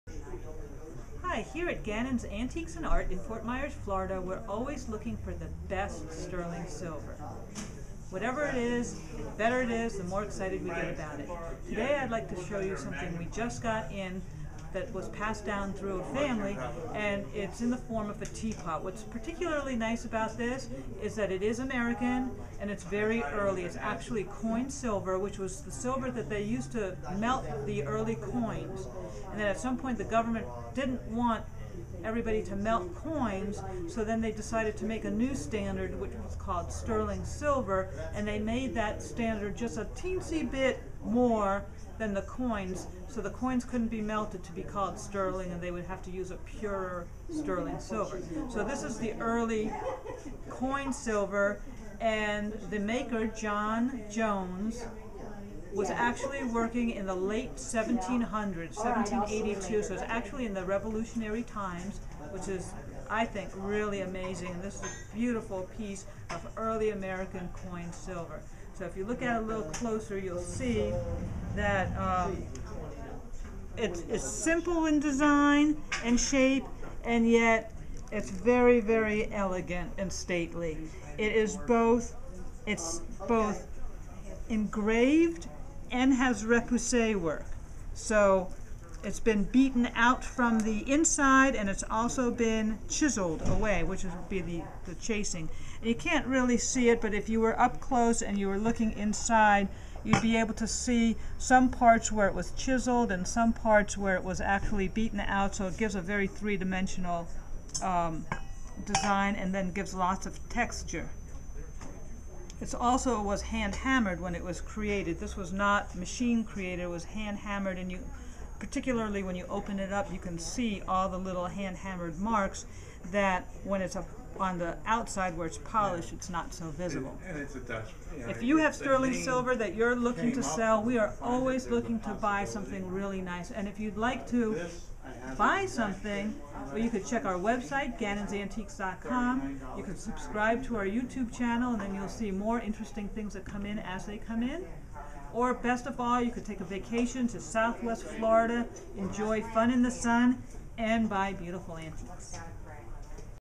Antique Silver, Antique coin silver tea pot c.1782, We buy silver
Antique coin silver tea pot predating sterling silver. From American silversmith John Jones c.1782. This beautiful antique silver tea pot is an antique from the Revolutionary Period. Its a great example of Early American Coin Silver. This hand hammered antique silver piece features engraving and repousse work. You can see this and many more amazing antique silver pieces in our antiques mall in Fort Myers, Florida where we buy silver and offer free antiques appraisals.